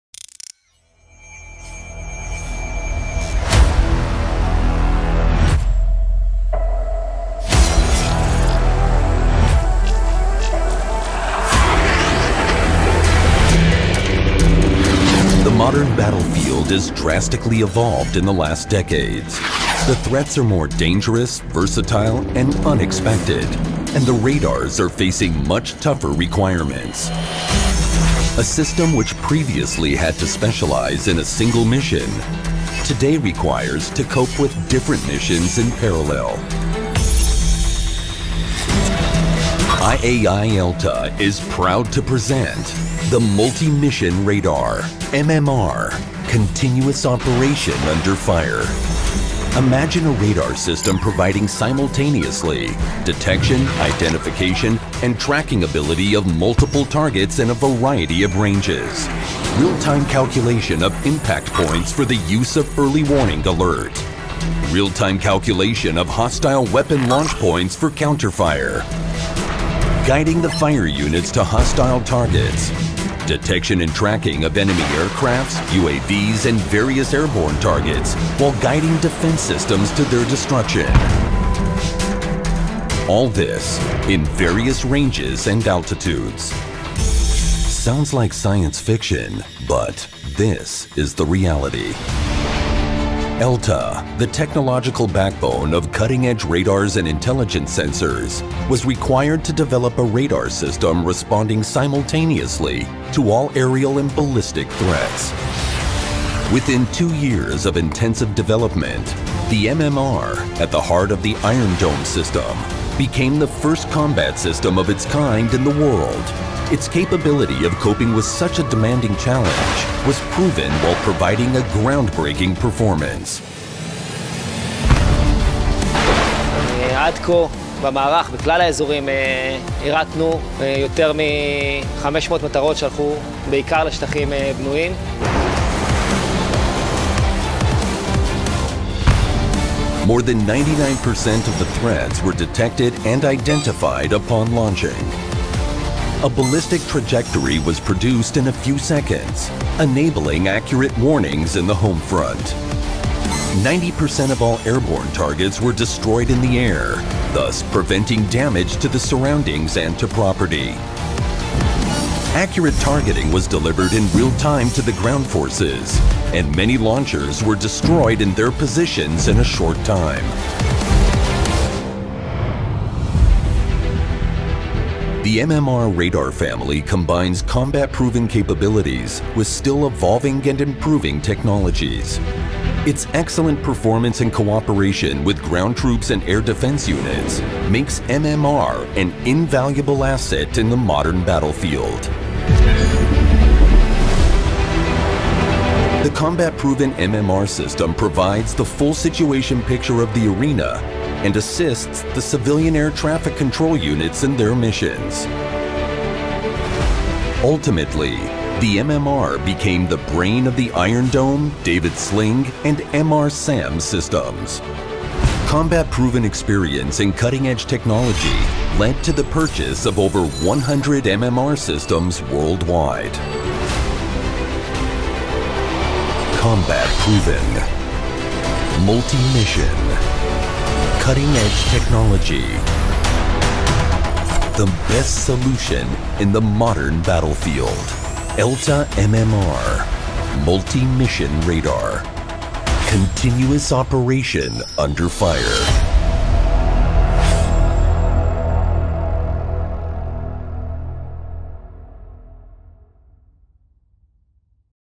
IAI-ELTA Systems Ltd. celebrates its 100th MMR being sold!
The ELM-2084 is a mobile S-Band Multi-Mission Radar (MMR) Family implementing an advanced 3D Active Electronically Steered Array (AESA) supporting modular and scalable architecture.
The MMR family supports Artillery Weapon Location & Air Defense operational missions and provides optimal solutions for short, medium and long range missions.
The ELM-2084 features high redundancy, graceful degradation, high reliability and very high availability.